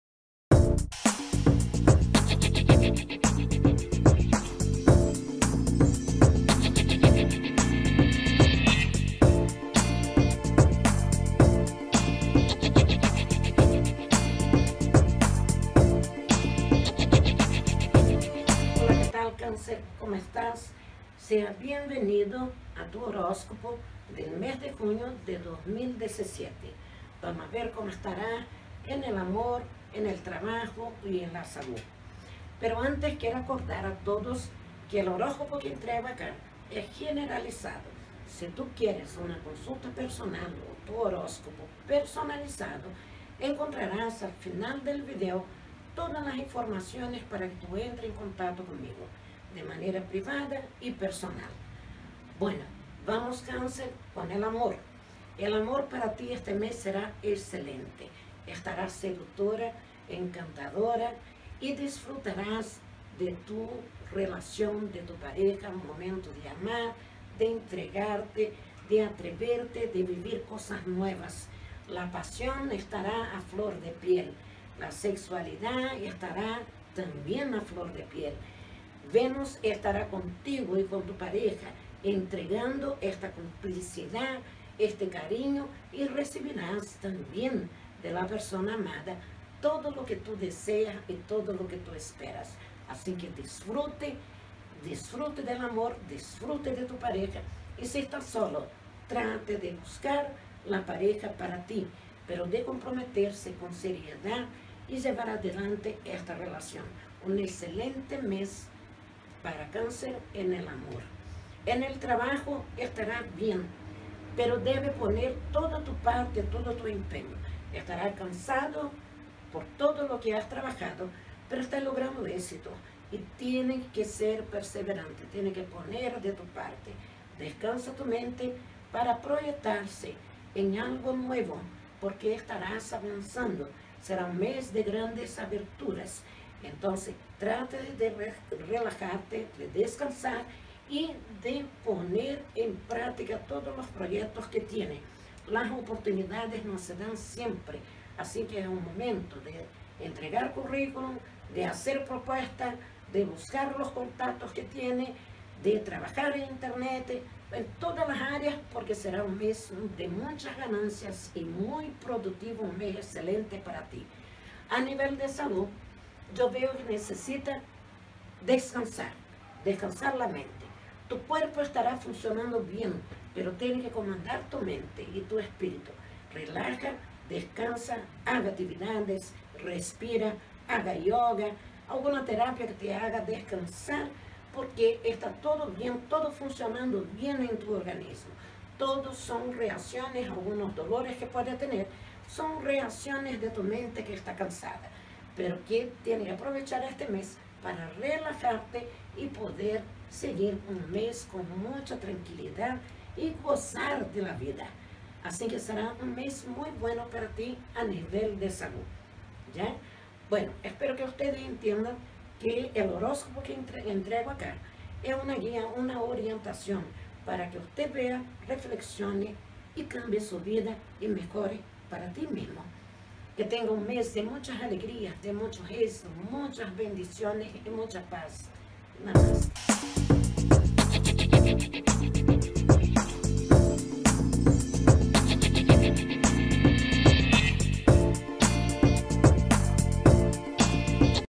CÁNCER JUNIO HORÓSCOPO 2017
En este vídeo entregare el horóscopo de Junio para el signo de Cáncer, hablare de amor, salud y trabajo. Entregando tips para que les valla super bien y puedan tener un excelente mes!!
¡Namaste!
Aplíquenlo!!!

Fatima Gomes Copyright ®.